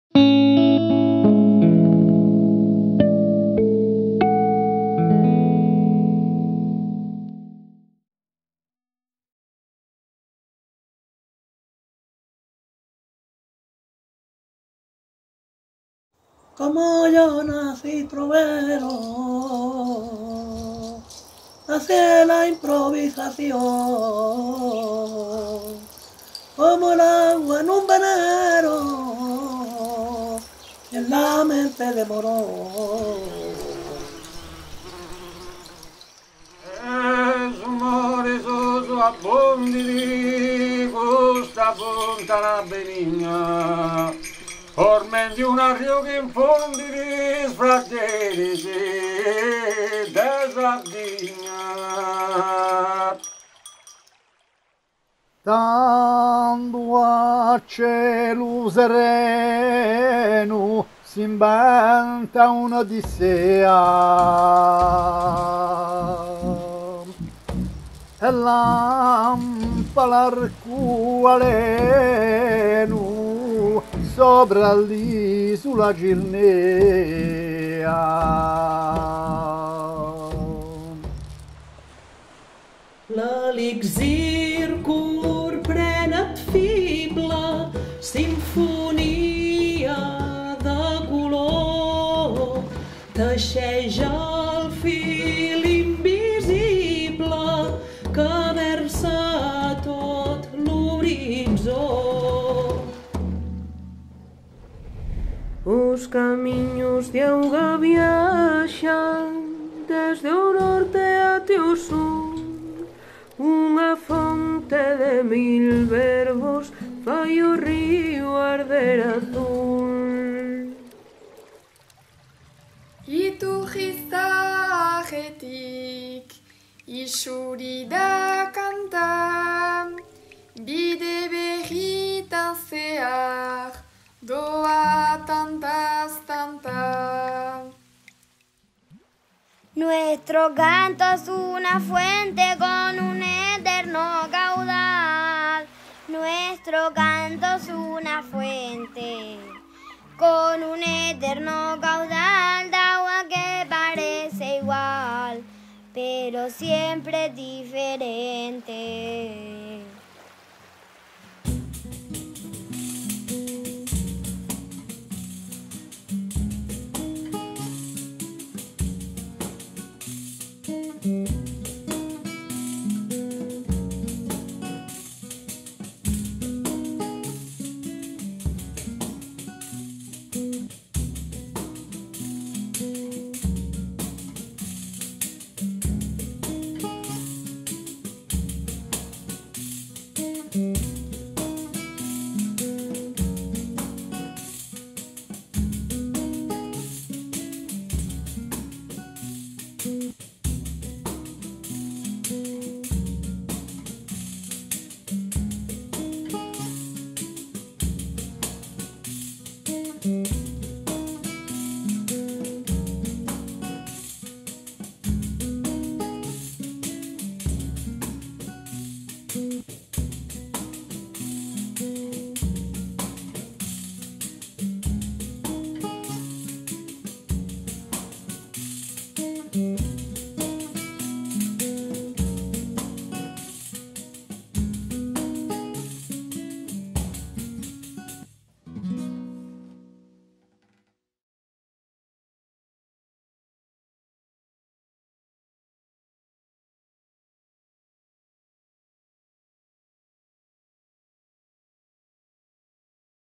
Bertsolaris, de la Alpujarra a Córcega
En este poema se encuentran siete corrientes de improvisación, como son la de los troveros, los repentistas, las regueifas, los payadores o los bertsolaris. Un poema creado por especialistas de la improvisación, desde la persona más mayor, de la Sierra de la Alpujarra, hasta una niña canaria repentista de siete años.

Esta pieza audiovisual, ilustrada por la dibujante gaditana Grillante y animada por Alfonso Benítez, retrata la tradición viva de la improvisación, un recorrido por el sur de Europa a través de un poema de 28 versos, construido y contado por siete voces diferentes, siguiendo cada poeta la estela de los versos anteriores, a modo de cadáver exquisito.

Ilustraciones: Grillante
Animación: Alfonso Benítez
Música, sonidos y efectos: Laia Pedrol & Marc Serrats
Asesoramiento: Karlos Aizpurua Etxarte & Alexis Díaz-Pimienta
Animación inicial: Mauro Entrialgo
Créditos finales: Xavi Olmos
Imagen gráfica: Alonso & Moutas
Naming y orientación poética: Peru Saizprez
Trovero: Juan Morón
Mutetu Campidanesu: Paulu Zedda
Poeta Corsica: Pierre Santucci
Glosa: Laia Pedrol
Regueifa/Brindo: Alba María
Bertsolari: Haira Aizpurua Ibargarai